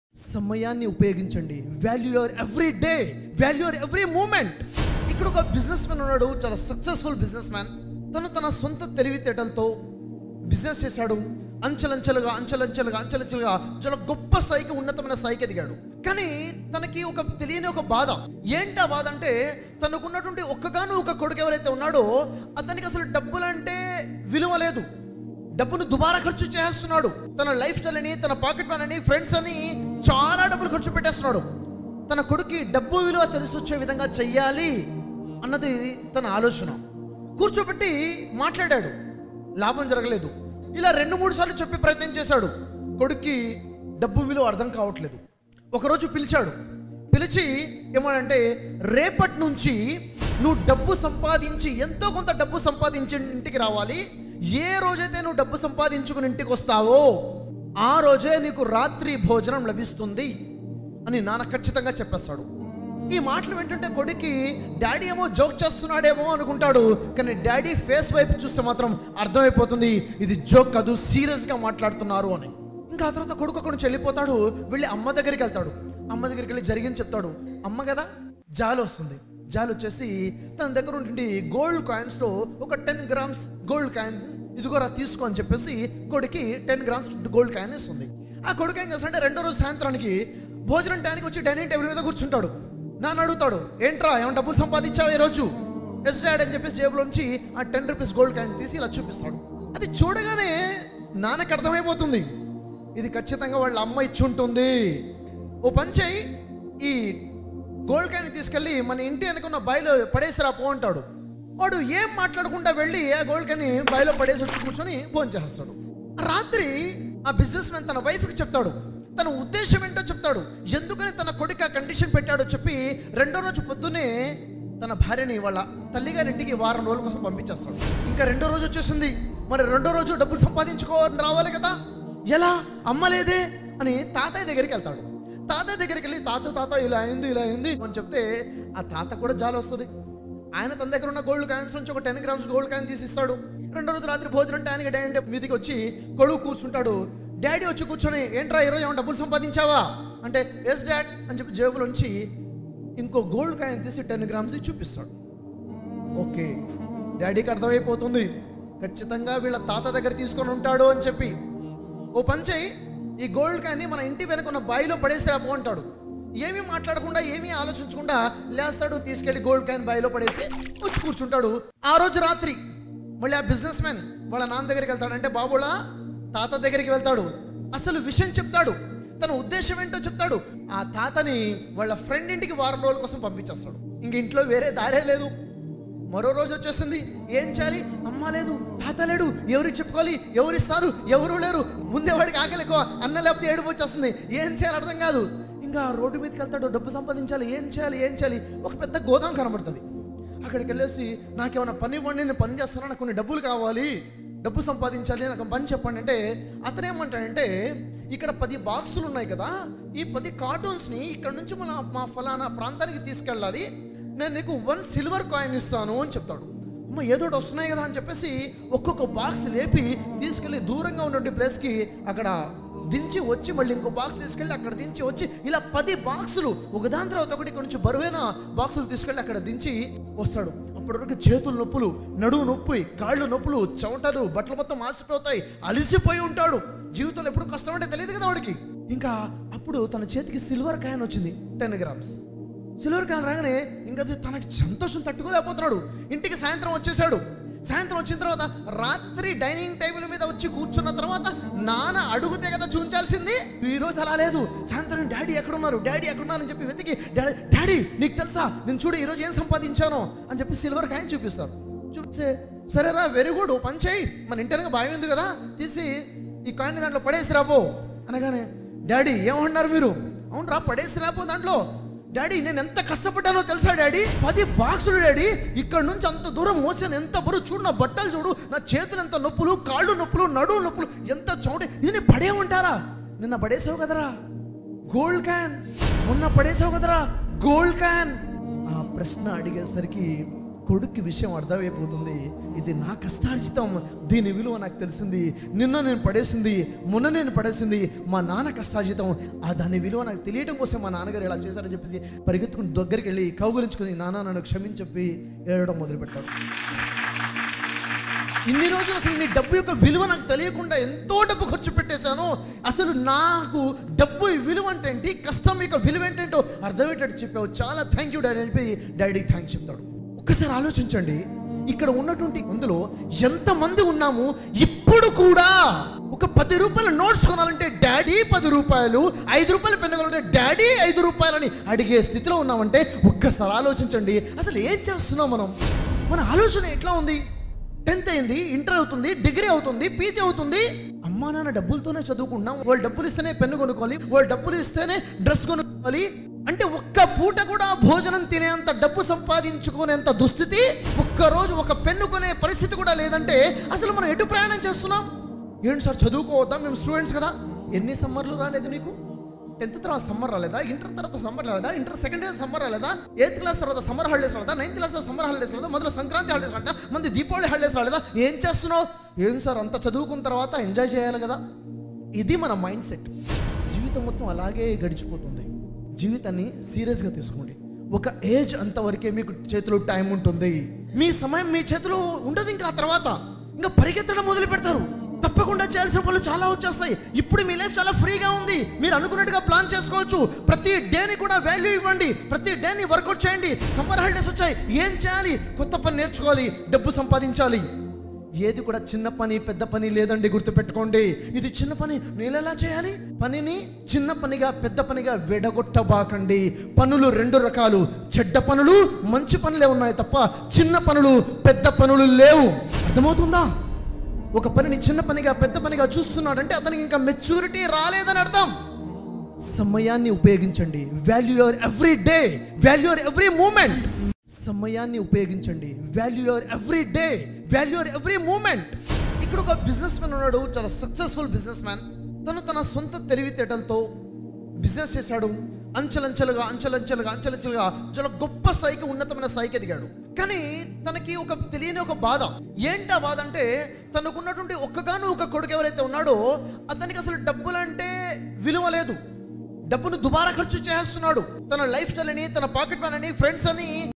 BUVVANAPALLI గ్రామం - PART - 1 - GANAPAVARAM మండలం - BR SHAFI డబ్బు విలువ తెలియజేయడం
BUVVANAPALLI గ్రామం - PART - 1 - GANAPAVARAM మండలం - BR SHAFI డబ్బు విలువ తెలియజేయడం // T.NAIDU MOTO VLOGS //